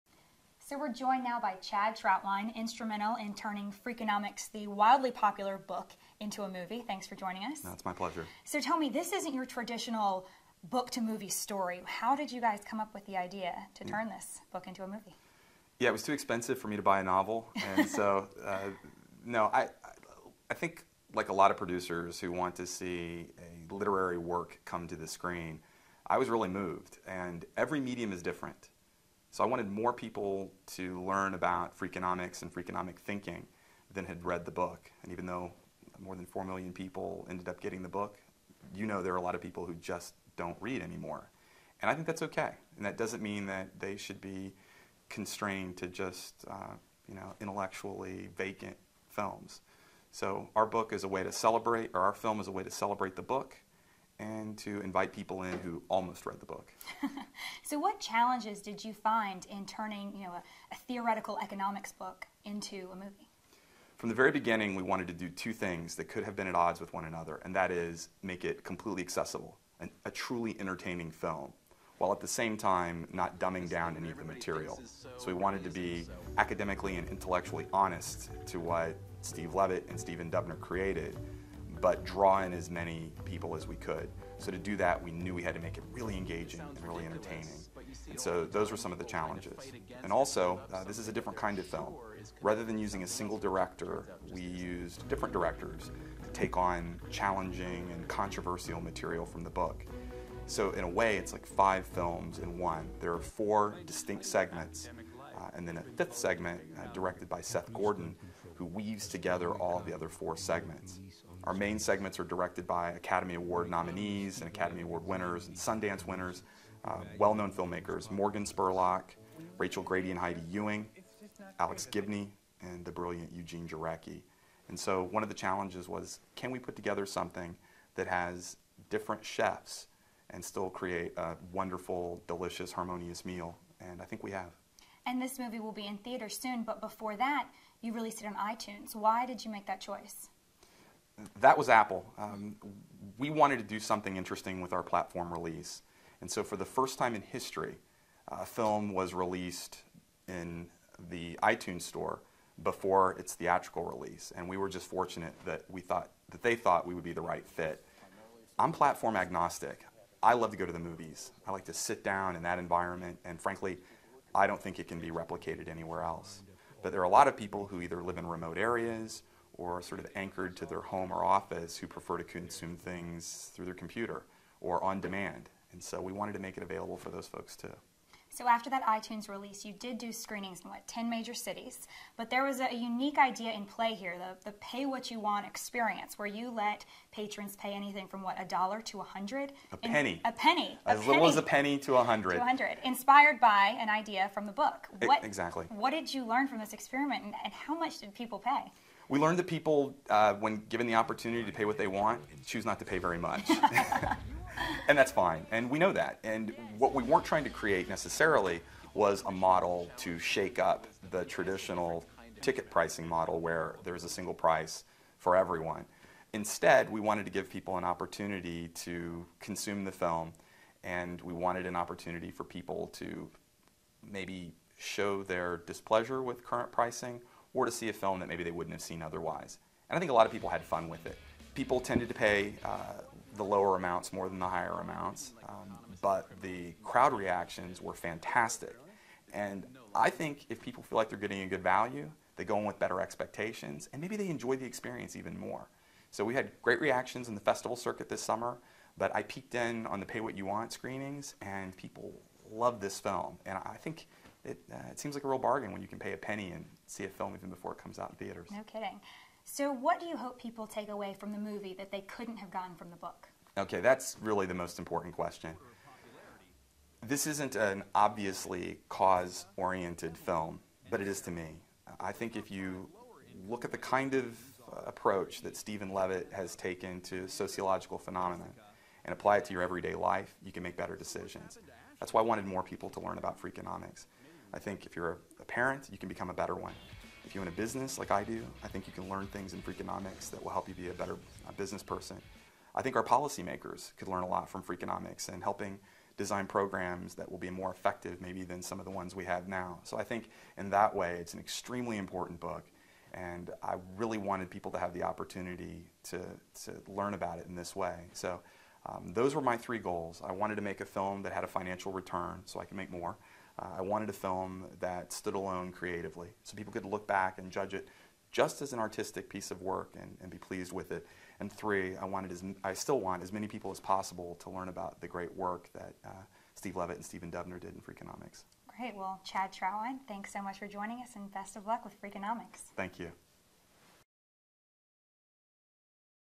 Merging Pop Culture & Economics at the Movies
When "Freakonomics," a book melding pop culture and economics, hit bookshelves in 2005 it was an instant best seller. With a production team of Alex Gibney, Morgan Spurlock, Rachel Grady and Heidi Ewing and Eugene Jarecki
"Freakonomics" is now hitting the big screen. CBS News Kaylee Hartung caught up with filmmaker Chad Troutwine for the DC premiere.